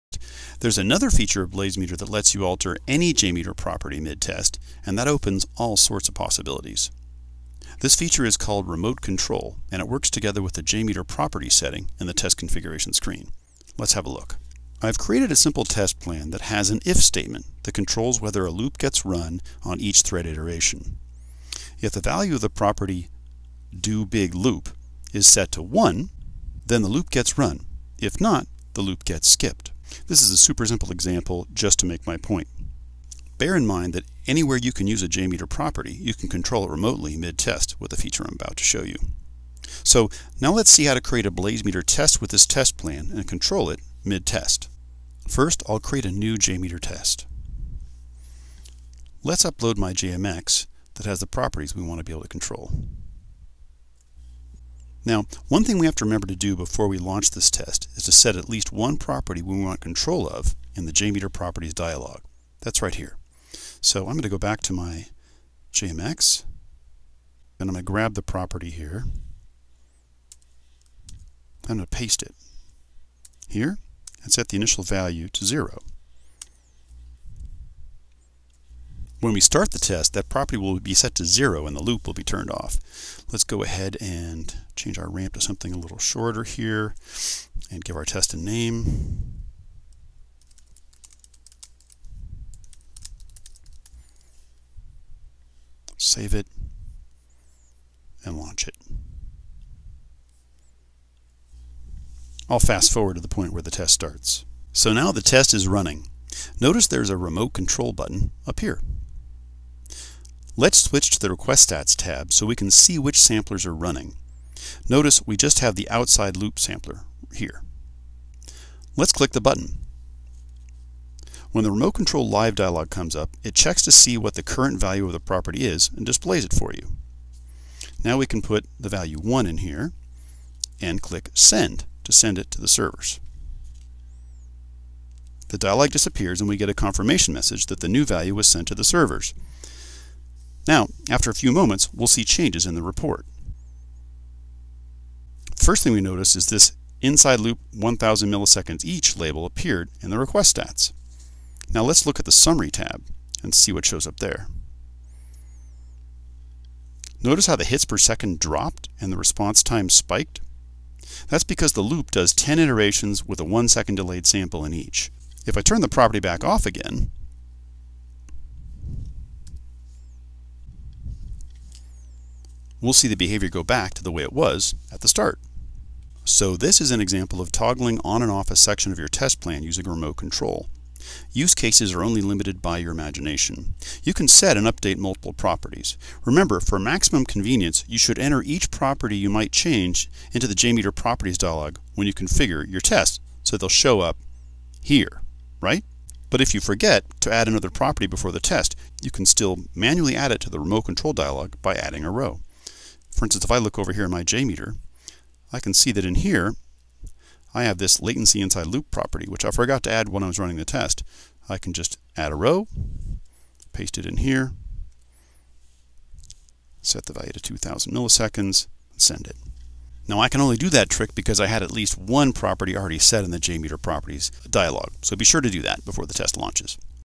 BlazeMeter Real Time JMeter Properties Remote Control
You can control any JMeter property mid-test with BlazeMeter's Remote Control feature. Be sure to enter at least one property in the JMeter Properties dialog on your test configuration screen before launching the test.